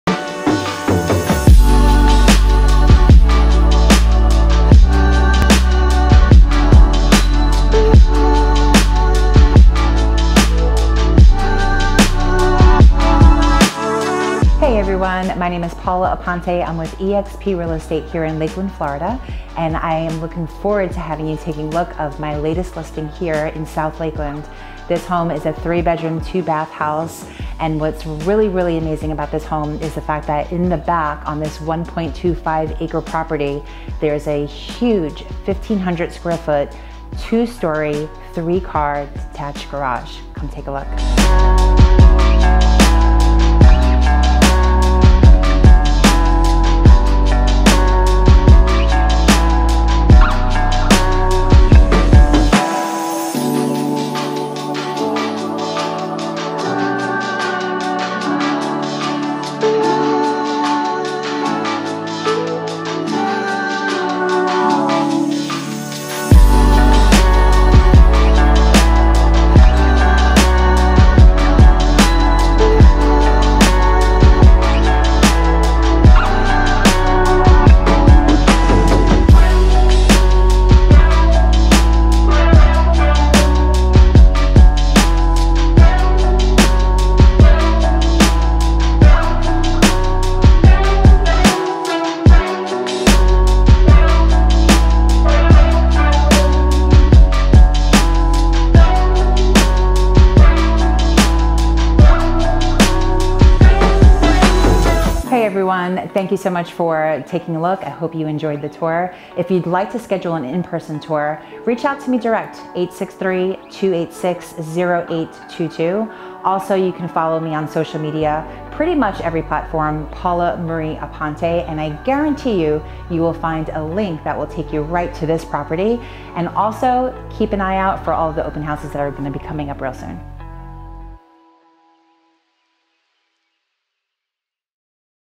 Beautiful South Lakeland Home For Sale on 1.25 acres!!
This South Lakeland home is situated on 1.25 acres and has NO HOA.  This 3/2 home has  1,864 sq feet of living space and includes an enclosed Florida room which is under air.  This home has plently of curb appeal with the professional river rock landscaping and concrete curbing along with an automatic watering system and fresh exterior paint.  The landscape lighting smart system was installed in 2022.  Enjoy the extra large screened in patio  (15x30) which leads out to an outdoor sitting (15x10)area over pavers which lead to the pool   The homes above ground swimming pool has been well maintained and features a beautiful private pool deck.  In addition to the 2 car attached garage you have plenty of additional space with 1,500 sq  feet inside of the detached 3 car garage/workshop which is 2 story and has its own loft area and loft windows. Both garages have automatic LED lights. There is also an RV pad with a 50 amp hook up. The home features a full security system, smart garage door opener on the main house. There are flood lights on both back parts of the house. The metal roof was completed in 2019 along with the new tankless hot water heater, duct work and AC return venting all completed in 2019.  Inside the home you have laminate planks throughout the living and bedroom areas, and  fresh interior paint.  This home is ready for you to move in and enjoy all the beauty this property has to offer.